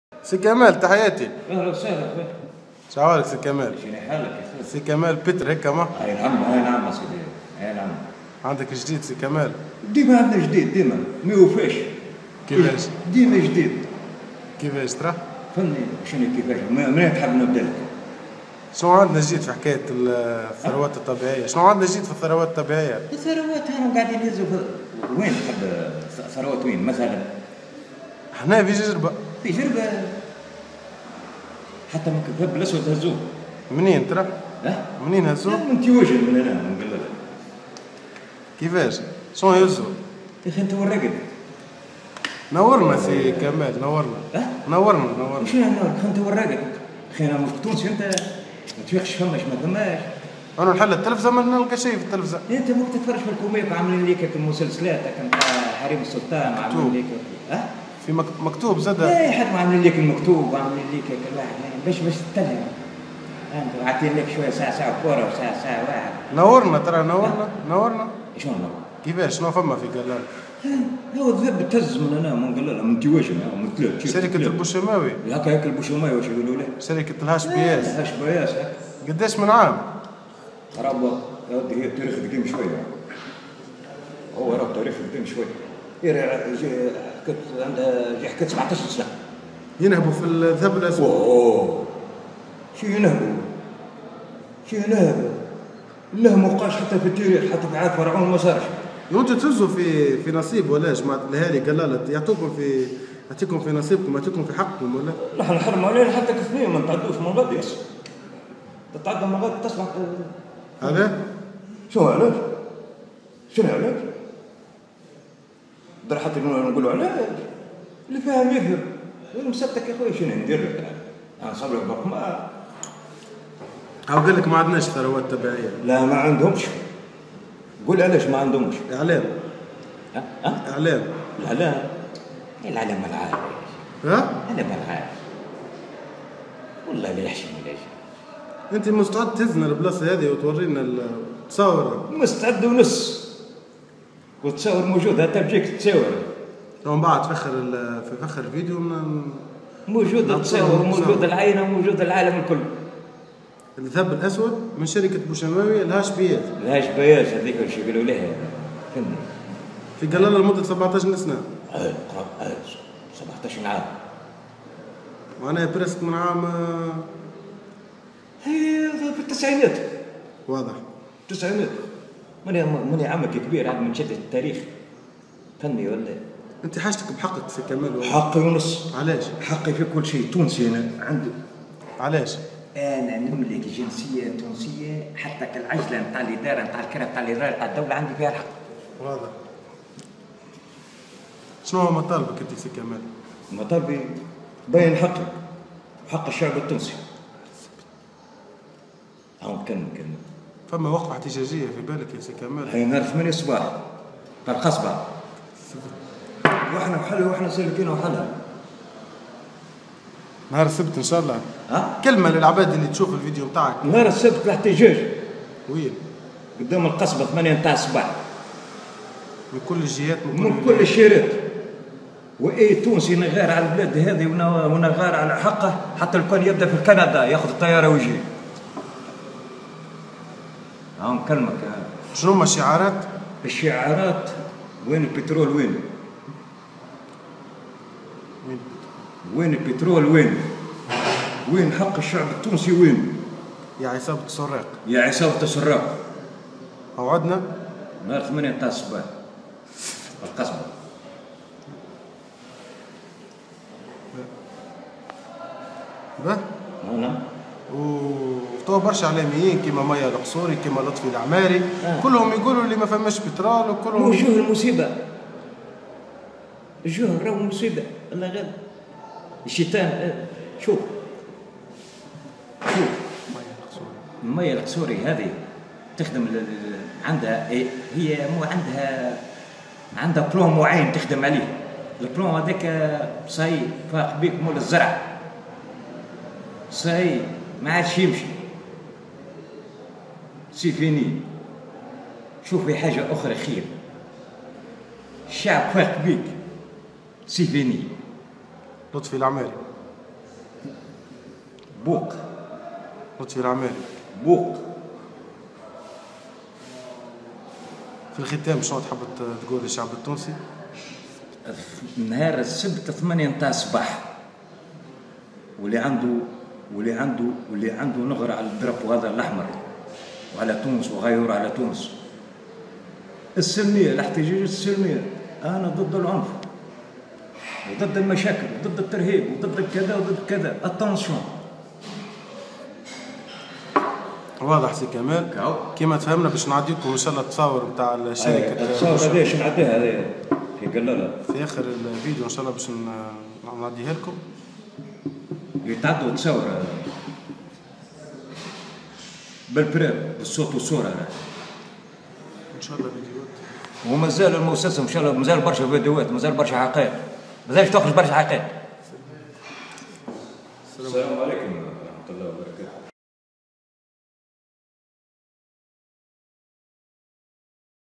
وينو_البترول كلمة حرة من مواطن تونسي
تصريح مهم من كمال
شوف و انشررررر